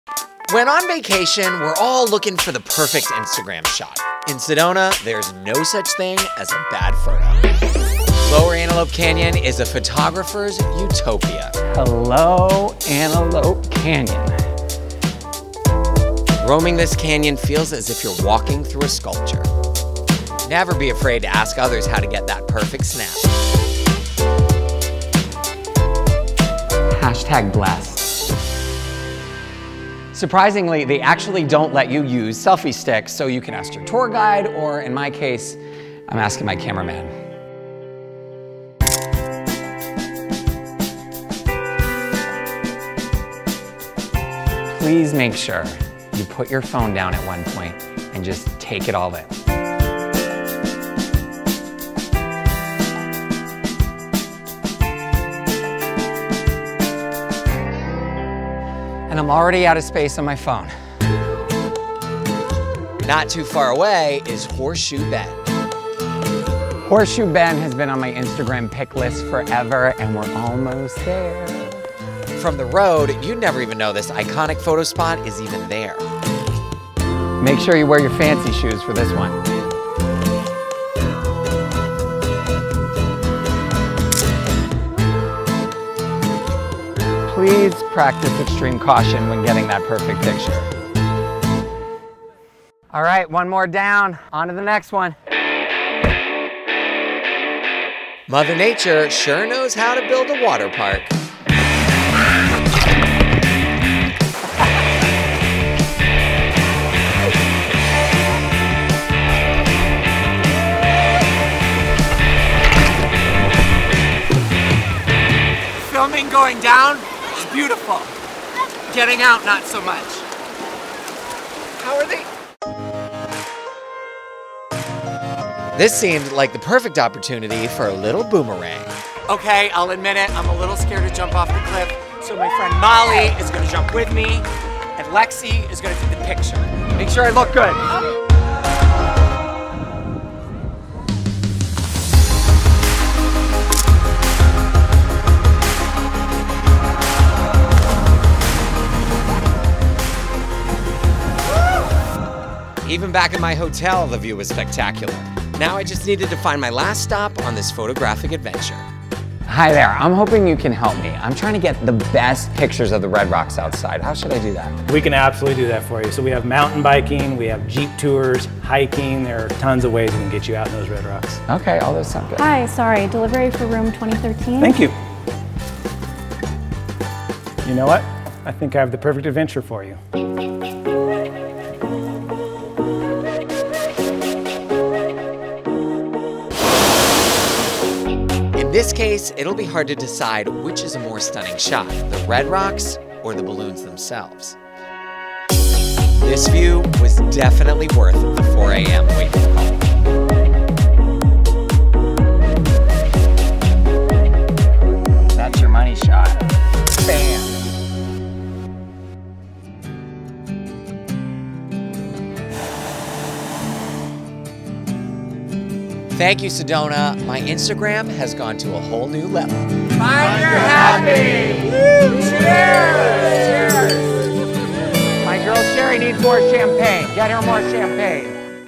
The World's Best Destination For Gorgeous Instagram Photos | Find Your Happy
We've found the perfect city to visit if you're looking to up your Instagram game — Sedona. This season, you don't have to travel far to fuel the jealousy and wanderlust in your friends. Of course, we'd never turn down the chance to explore Kauai or Venice, but there are plenty of alluring — and yes, highly photogenic — destinations right under your nose. One of our favorites? Sedona, Arizona. Between the breathtaking view of the Colorado River from Horseshoe Bend and the other-worldly rock formations of Lower Antelope Canyon, there are plenty of sights to explore — ones that will rack up the Instagram likes. Follow along with our host Matthew Rodrigues to discover the four spots you can't miss.

We've partnered with Hilton Hotels & Resortsto show you why Sedona is the perfect destination for your next photographic adventure.